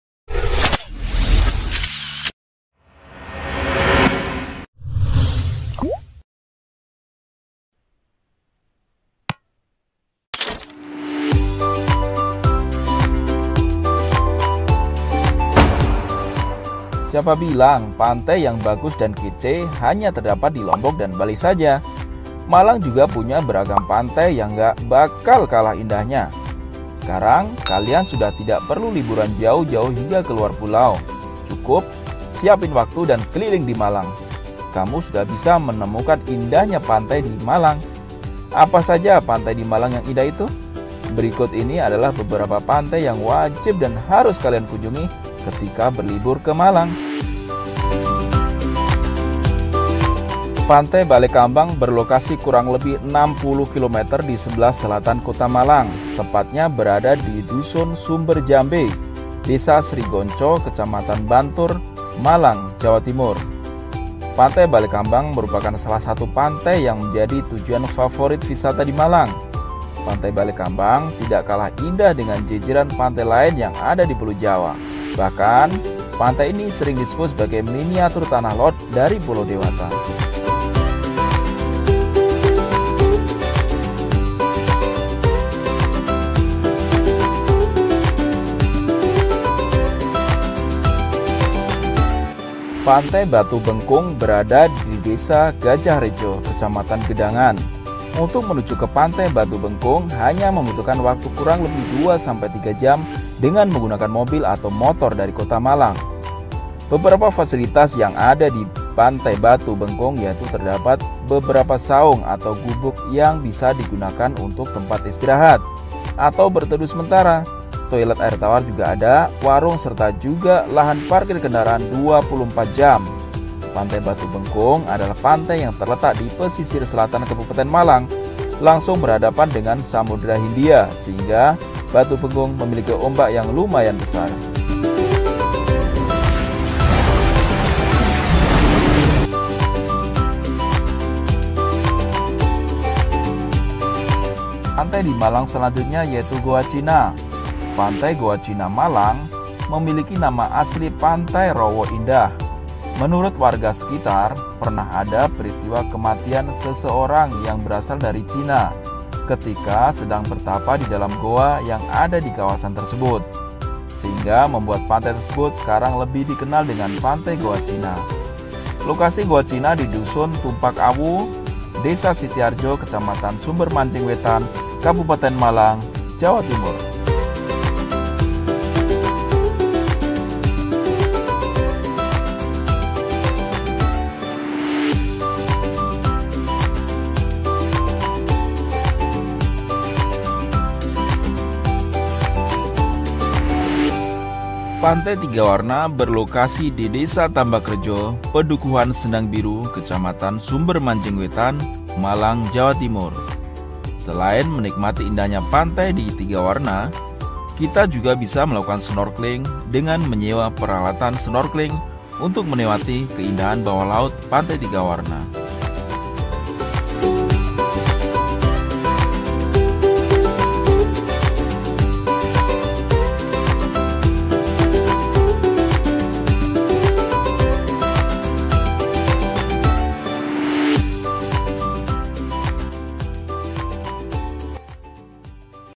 4 PANTAI KEREN DI MALANG YANG WAJIB DIKUNJUNGI
Siapa bilang, pantai yang bagus dan kece hanya terdapat di Lombok dan Bali saja? Malang juga punya beragam pantai yang nggak bakal kalah indahnya, lho. Sekarang, kalian sudah tidak perlu liburan jauh-jauh hingga ke luar pulau, cukup siapin waktu dan keliling di Malang saja, kamu sudah bisa menemukan indahnya surga-surga dunia di bawah ini.

Apa saja pantai di Malang yang indah? berikut ini adalah beberapa pantai yang wajib dan harus kalian kunjungi ketika berlibur ke Malang

1. Pantai Balekambang
Pantai Balekambang berlokasi kurang lebih 60 km di sebelah selatan kota Malang, tepatnya berada di Dusun Sumber Jambe, Desa Srigonco, Kecamatan Bantur, Malang, Jawa Timur.

Pantai balekambang merupakan salah satu pantai yang menjadi tujuan favorit wisata di Malang, pantai Balekambang tidak kalah indah dengan jejeran pantai lain yang ada di pulau jawa, bahkan pantai ini sering disebut sebagai Miniatur Tanah Lot dari Pulau Dewata.

2. Pantai Batu Bengkung
BATU BENGKUNG

Pantai Batu Bengkung berada di Desa Gajah Rejo, Kecamatan Gedangan, untuk menuju ke pantai Batu Bengkung hanya membutuhkan waktu kurang lebih 2-3 jam dengan mengunakan mobil atau motor dari Kota Malang.

Beberapa fasilitas yang ada di pantai Batu Bengkung yaitu terdapat beberapa saung / gubuk yang bisa digunakan untuk tempat istirahat atau berteduh sementara, toilet air tawar, warung, serta juga lahan parkir kendaraan 24 jam. pantai Batu Bengkung adalah pantai yang terletak di pesisir selatan Kabupaten Malang. Langsung berhadapan dengan Samudera Hindia sehingga Batu Bengkung memiliki ombak yang lumayan besar.

Refference:
-Foto foto dari berbagai sumber